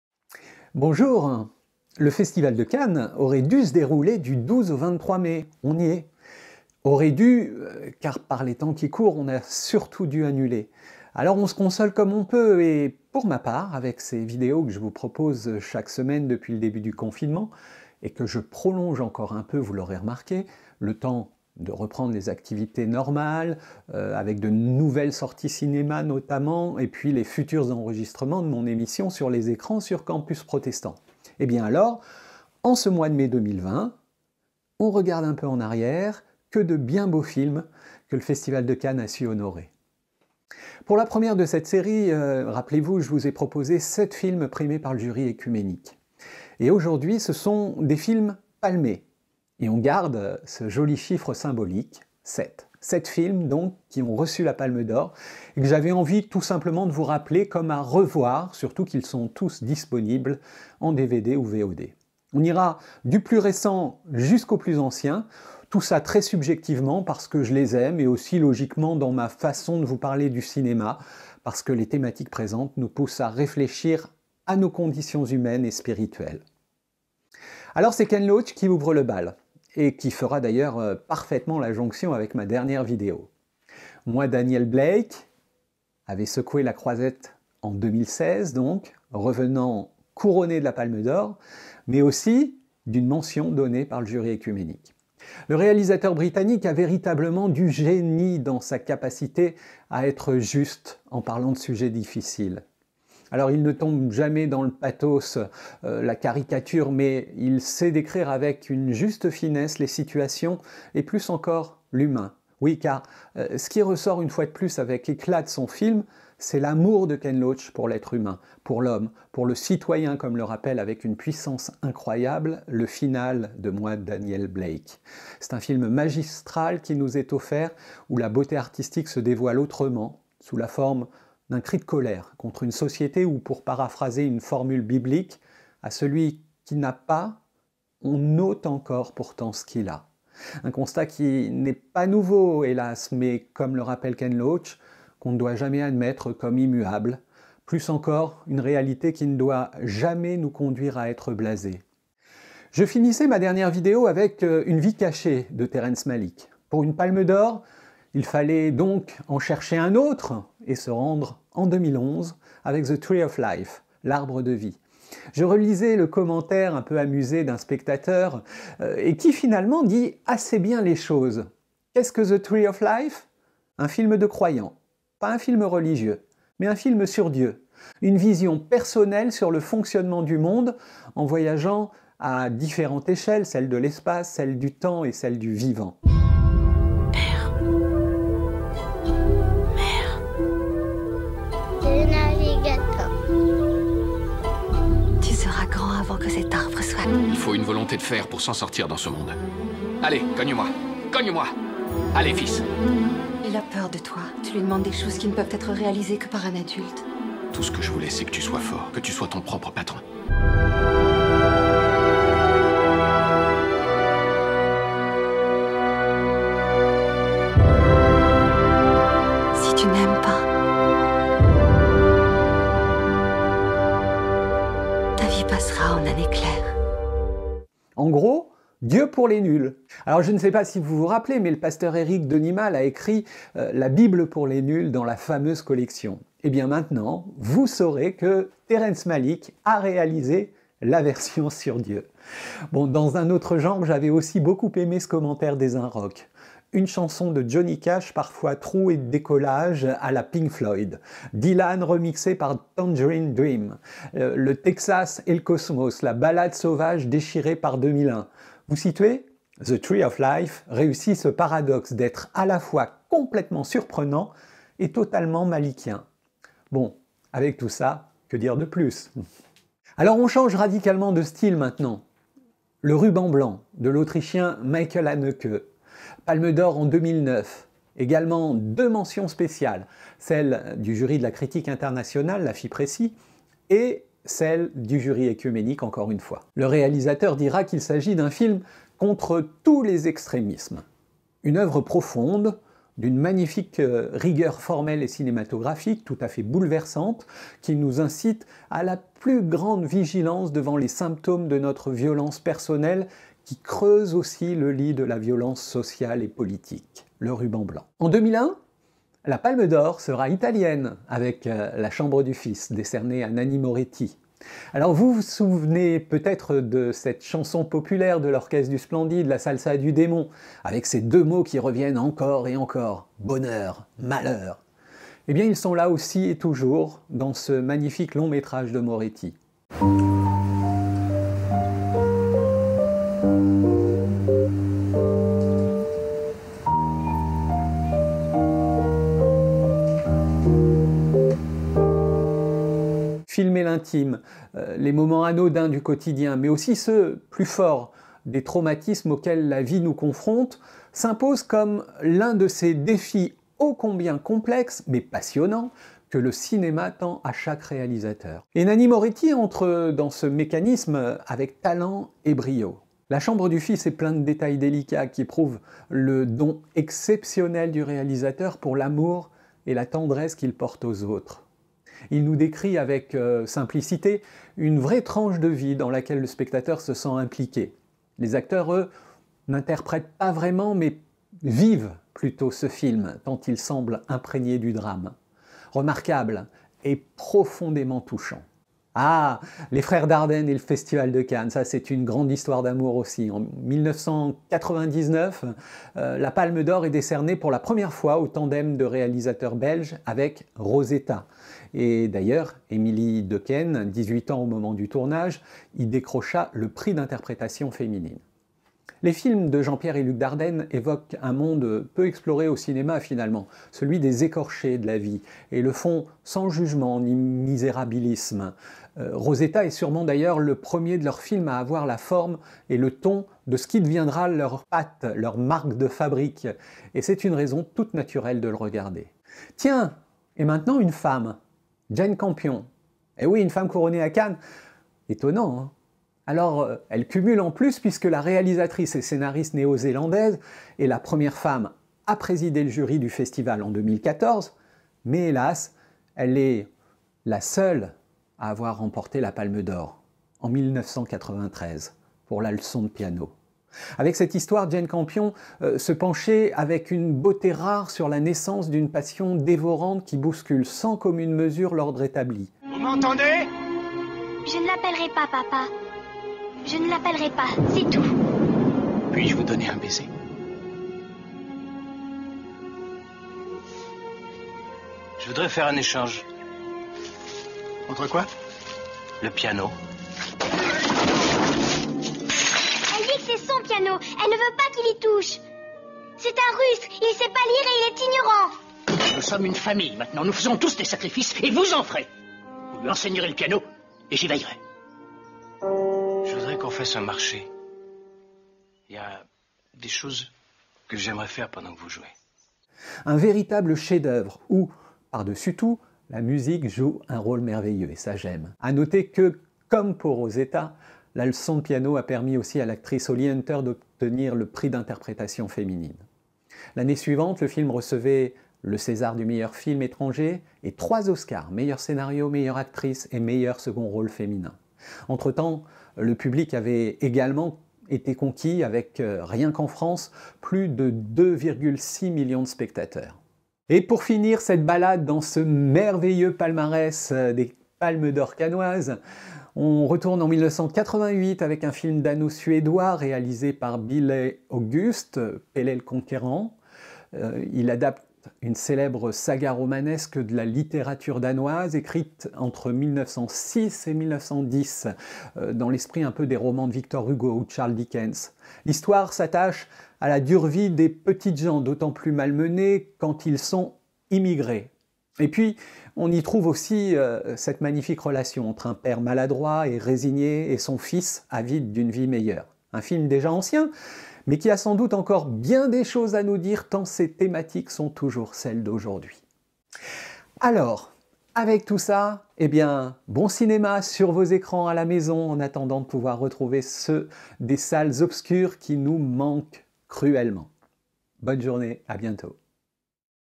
7 films palmés
Dans le cadre de mes "vidéos confinement", et en ce mois de mai pendant lequel devait se dérouler le Festival de Cannes, je vous propose quelques films en lien avec le Festival. Cette semaine, une sélection de 7 films qui ont reçu la palme d'or.